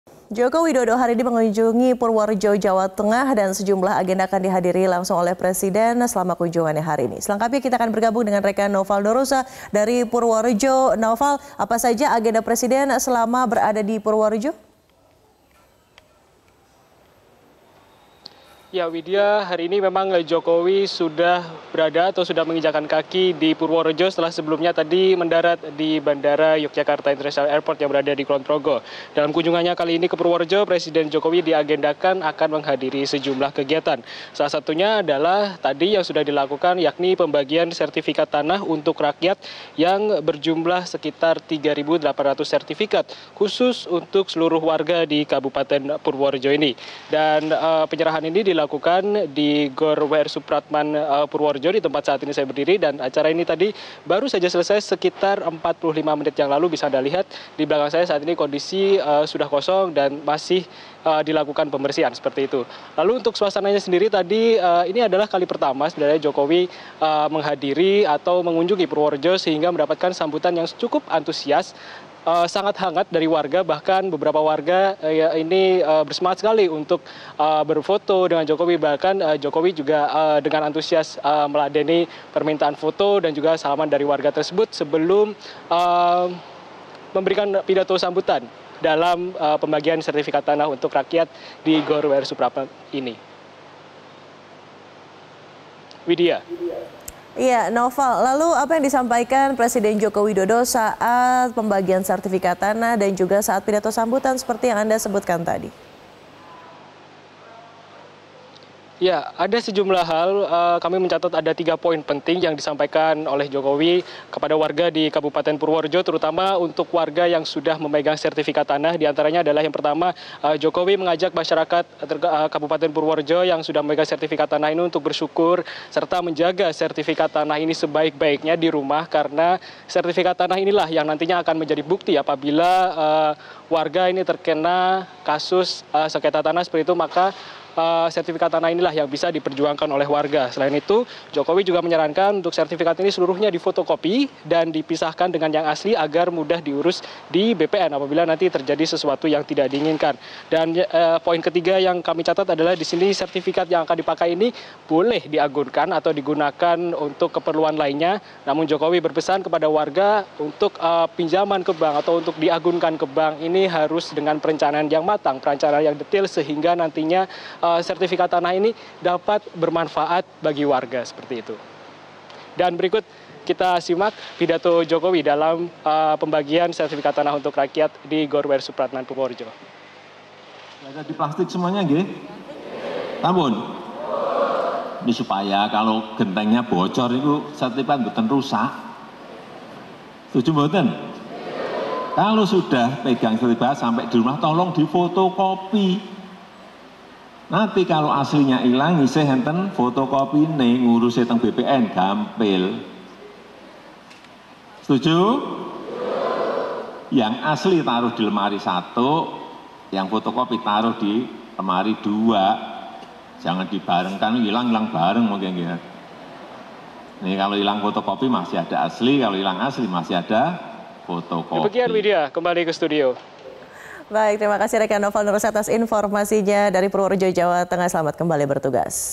Jokowi Bagikan 3800 Sertifikat Tanah untuk Warga Purworejo
Presiden Joko Widodo menyerahkan 3.800 sertifikat tanah untuk warga Kabupaten Purworejo, Jawa Tengah. Pembagian sertifikat tersebut merupakan bagian dari program percepatan penerbitan sertifikat tanah untuk rakyat.